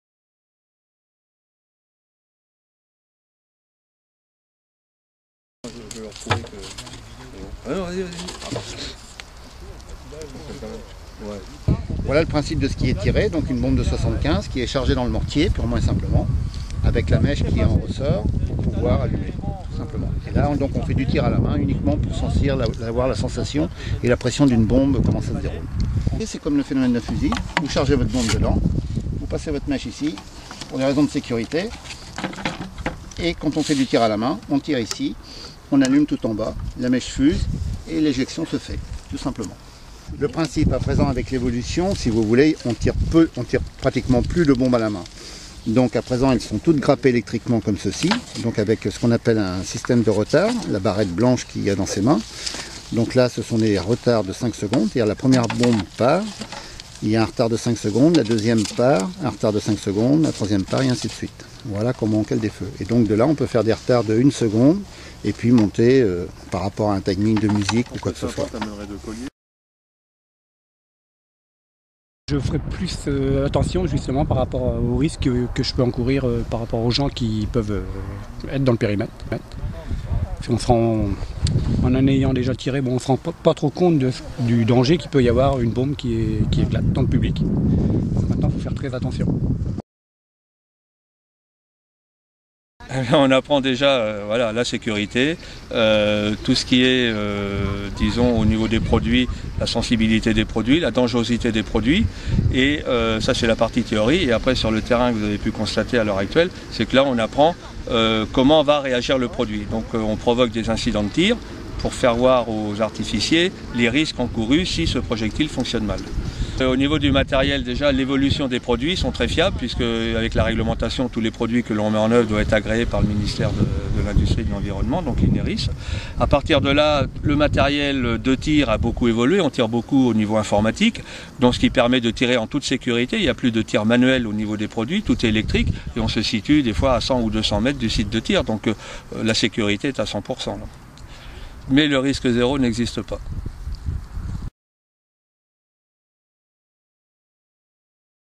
Les artificiers formés et prêts pour le 14 Juillet
Quinze hommes, majoritairement des agents communaux, ont suivi un stage d'artificier professionnel, à Contres, du 14 au 18 novembre. Cette formation et l'obtention d'un diplôme seront bientôt obligatoires pour tirer un feu d'artifice. Durant cette semaine, leur maître de stage était Claude Devernois, metteur en scène et technicien en chef de la réalisation du feu du 14 Juillet dernier, à Paris.